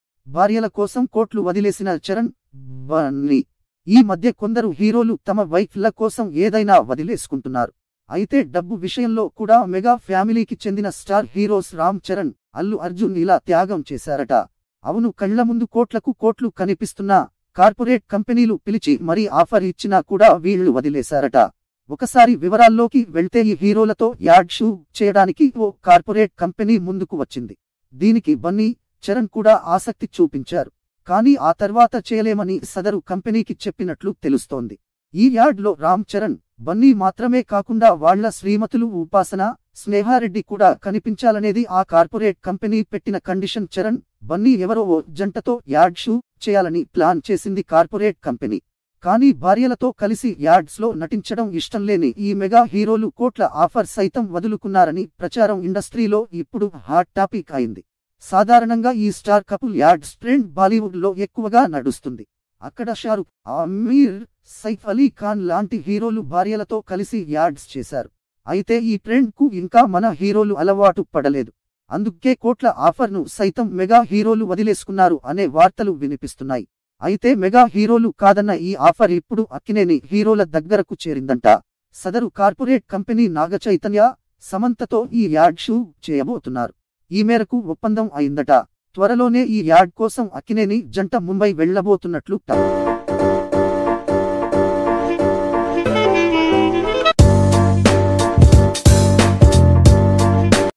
భార్యల కోసం కోట్లు వదిలేసిన చరణ్, అర్జున్... ఎందుకో తెలుసా
Allu Arjun and Ram Charan left the deal of crores because of their wives UPASANA and Sneha Reddy